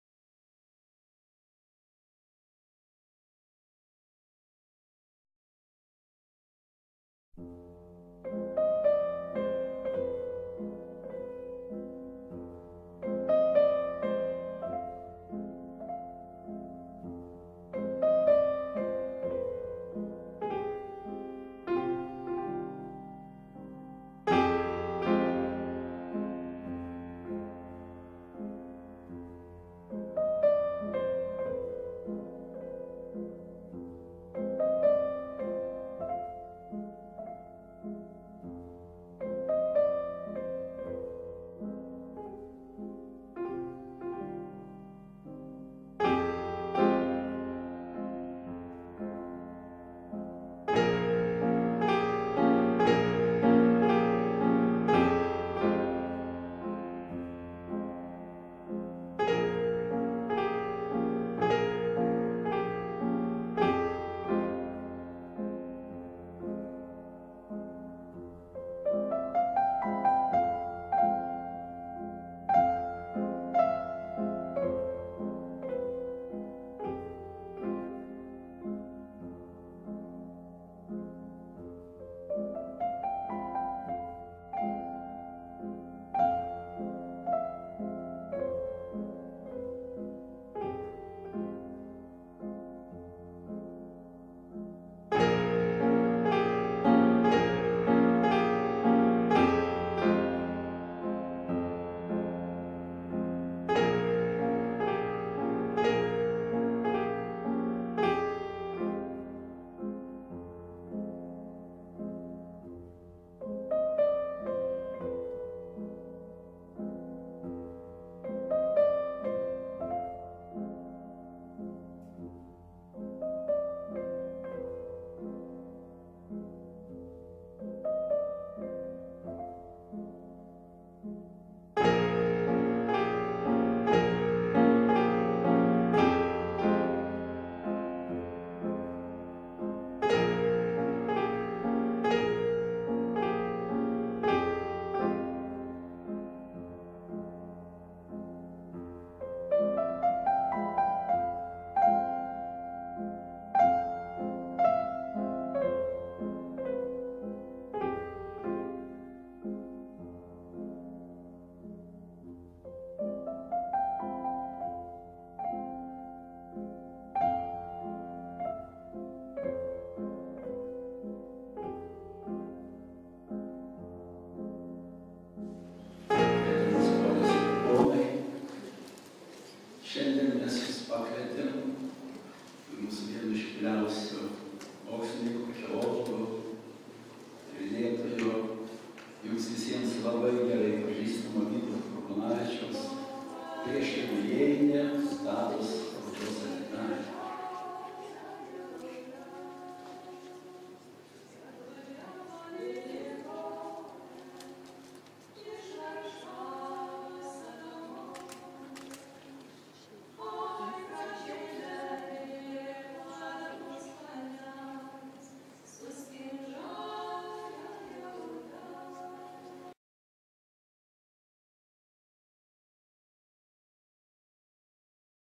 Archeologo habil. dr. Vytauto Urbanavičiaus darbų paroda „Sugrąžinta praeitis"
Žymaus Lietuvos archeologo habil. dr. Vytauto Urbanavičiaus darbų parodoje „Sugrąžinta praeitis" eksponuojami mokslininko tyrinėti paminklai, kuriems jis skyrė daugiau kaip 50 metų.